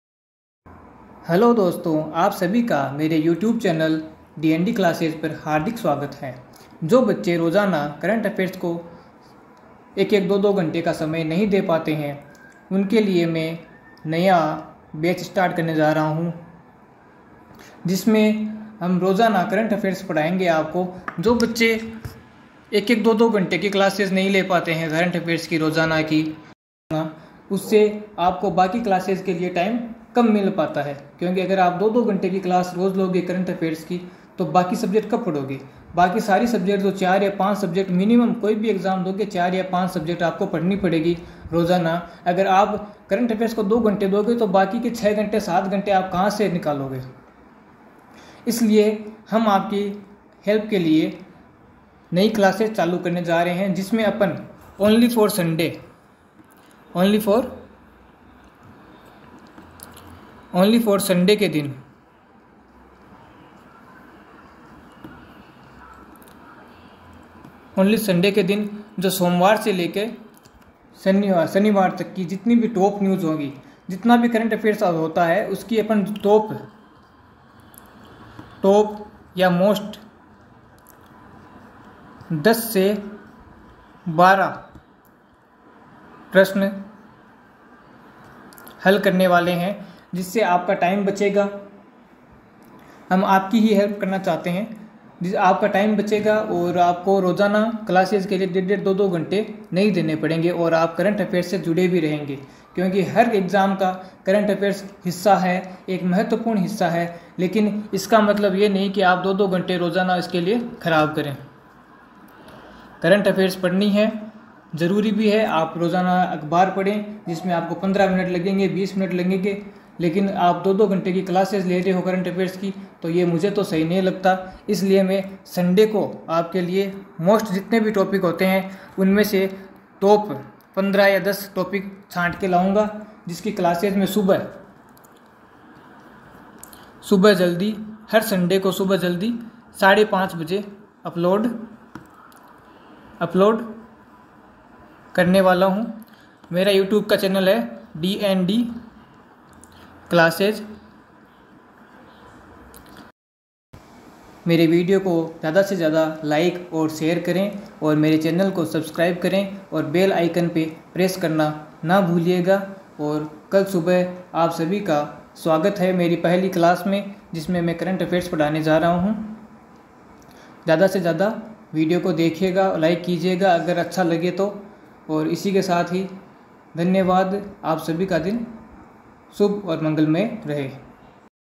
Current Affairs Weekly all sunday weekly 15 most Current news
हम प्रत्येक रविवार सुबह 5.30 बजे आपके लिए पूरे सप्ताह की करंट अफेयर्स के प्रमुख 15 प्रश्न की चर्चा करने वाले है वो भी कम से कम समय में, 15 से 20 मिनट अधिकतम ।
जो विद्यार्थी रोजाना करंट अफेयर्स को ज्यादा समय नहीं दे पाते है उन सब के लिए यह क्लास फायदेमंद साबित होगी ये आपको करंट अफेयर्स से जोड़े रखेगी और इसके अलावा जो रोजाना करंट अफेयर्स पढ़ते है उनके लिए यह Revision Booster सिद्ध होगी ।

निरंतर Classes or update पाने के लिए  bell 🔔  Icon पर Press करे ।

Thankyou.